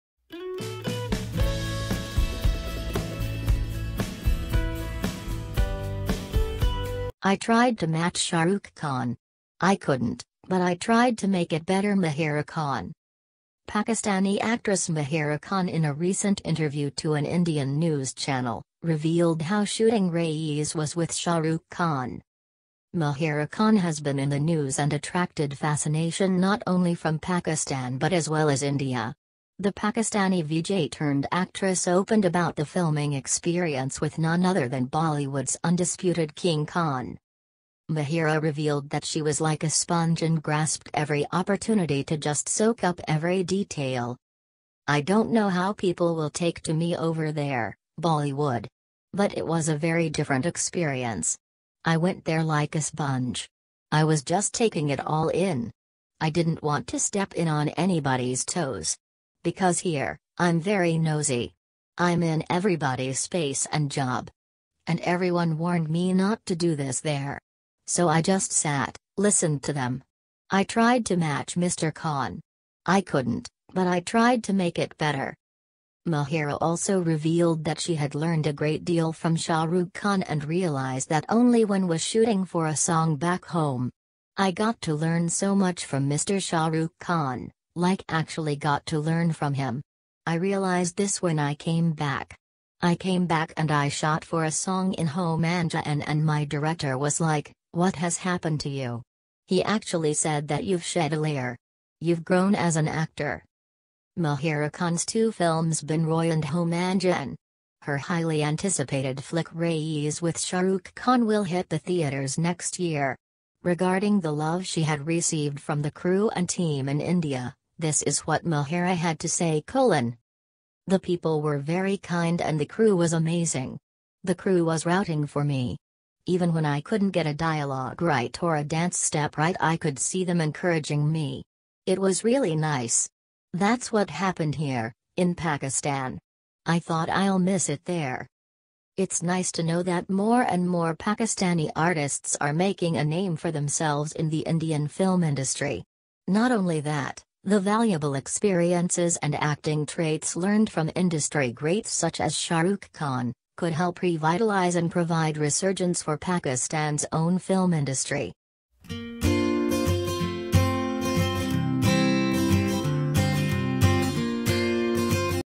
Mahira Khan : I tried to match Shah Rukh Khan
"I tried to match Mr Khan. I couldn't, but I tried to make it better," said Mahira

Pakistani actress Mahira Khan is all set for her first steps in Bollywood and with no less than Shah Rukh Khan, in Rahul Dholakia's Raees. The actress, who became a household name in India after her stint in Pakistani TV show Humsafar with actor Fawad Khan (who made an impressive Bollywood debut in Khoobsurat last year) talked about her foray in the Hindi film industry and her films back home - Bin Roye and Ho Mann Jahaan.